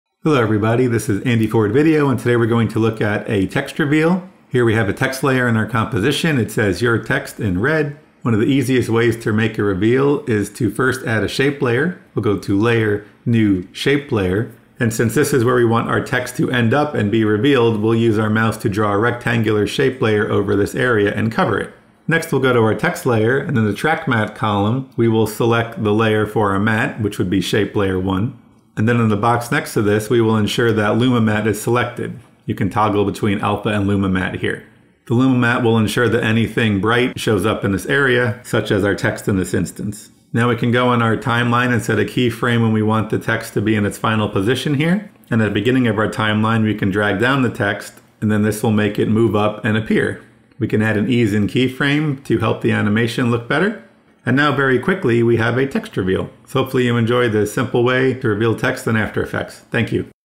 After Effects Tutorial - Easy Text Reveal
If you want a text layer to be revealed, an easy method is to use a shape layer. Create the shape layer so that it covers the area you want your text to fully appear. Then, go to your text layer’s Track Matte column. Set the matte to be the shape layer. Then, ensure it is set to Luma Matte (using the box next to the matte layer dropdown). With Luma Matte selected, anything “bright” will show up in the shape layer area… such as your text.

Next, set position keyframes for the text layer so that it moves into your final position and is revealed.